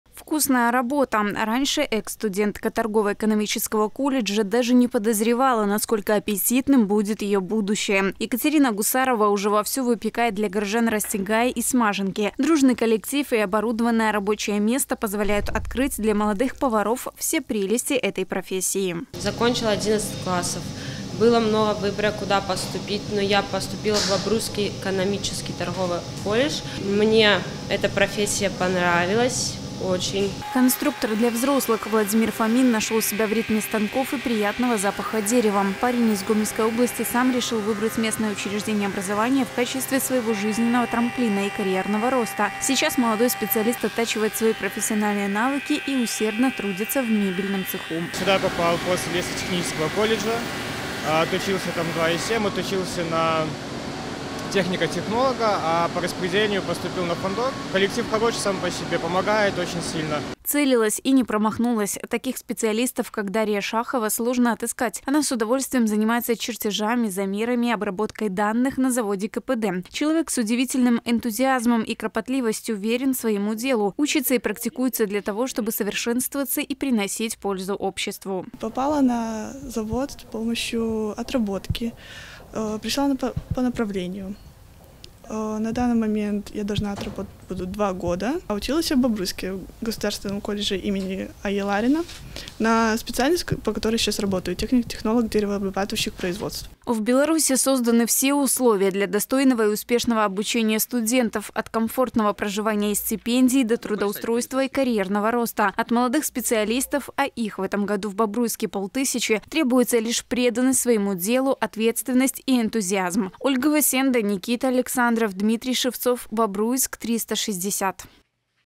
Место найдётся всем
С первого августа на рабочие места по всей стране прибыли молодые специалисты.